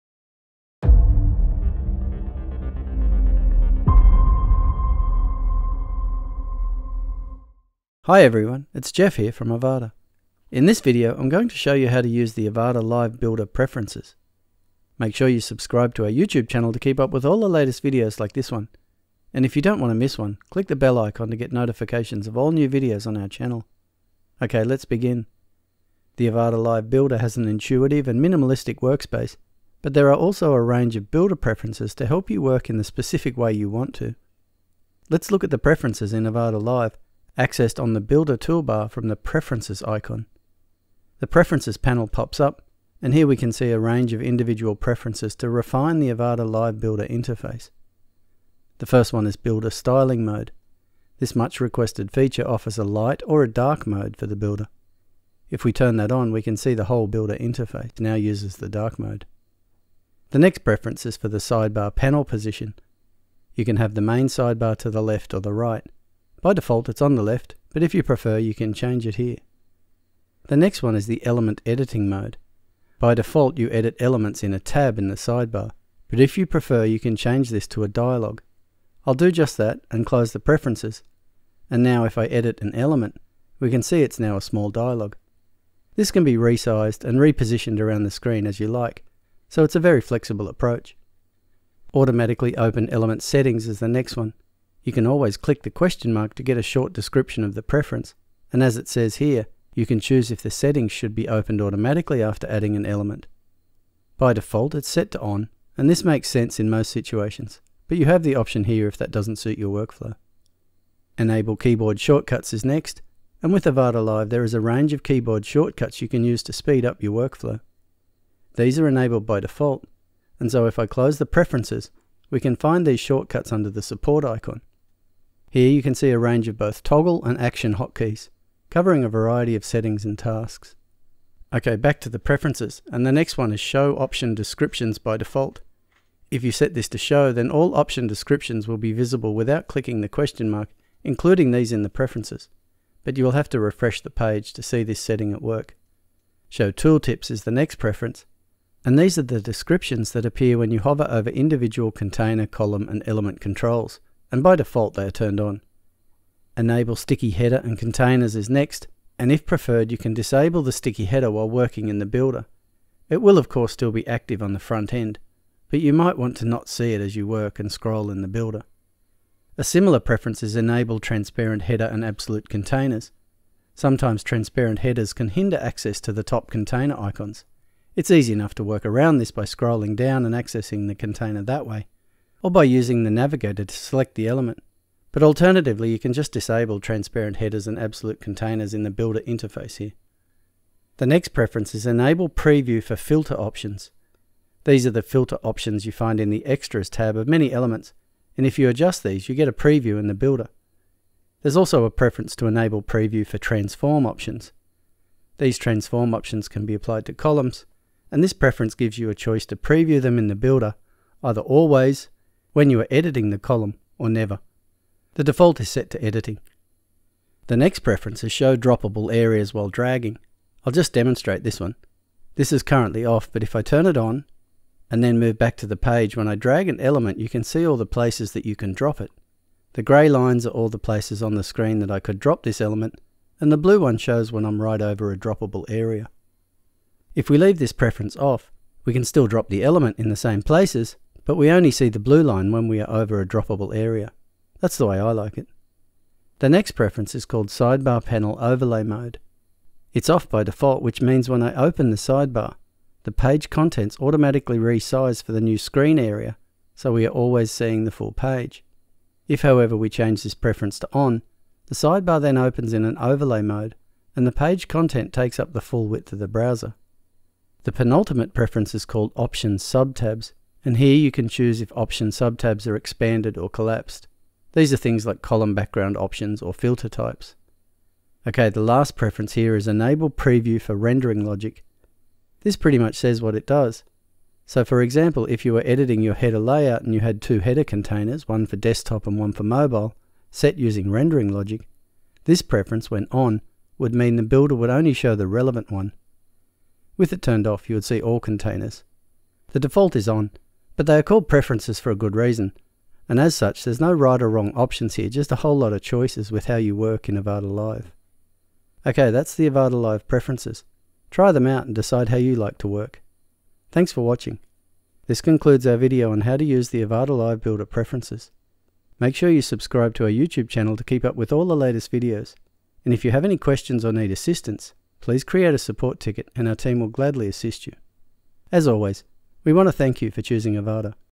How to Use the Avada Live Builder Preferences
This video tutorial explains how to use the Avada Live builder preferences accessed from the main toolbar, allowing users to tailor their workspace to their liking:

✓ - *Builder Styling Mode:* Switch between a light or dark mode for the builder interface.
✓ - *Sidebar Panel Position:* Choose the sidebar's position on the left or right.
✓ - *Element Editing Mode:* Select between editing elements in a sidebar tab or a dialog box.
✓ - *Automatically Open Element Settings:* Choose if settings should open automatically after adding an element.
✓ - *Enable Keyboard Shortcuts:* Access a variety of shortcuts to speed up workflow.
✓ - *Show Option Descriptions by Default:* Option descriptions are visible without clicking the question mark.
✓ - *Show Tooltips:* Enable or disable tooltips for container, column, and element controls.
✓ - *Enable Sticky Header and Containers:* Decide whether to show sticky headers while working in the Builder.
✓ - *Enable Transparent Header and Absolute Containers:* Choose whether to see transparent headers in the Builder.
✓ - *Enable Preview for Filter and Transform Options:* Toggle preview options for element filters and column transformations.
✓ - *Show Droppable Areas While Dragging:* Visualize potential drop locations for elements.
✓ - *Sidebar Panel Overlay Mode:* Choose a resizing or overlay mode for the sidebar.
✓ - *Options Subtabs:* Decide if option subtabs are expanded or collapsed by default.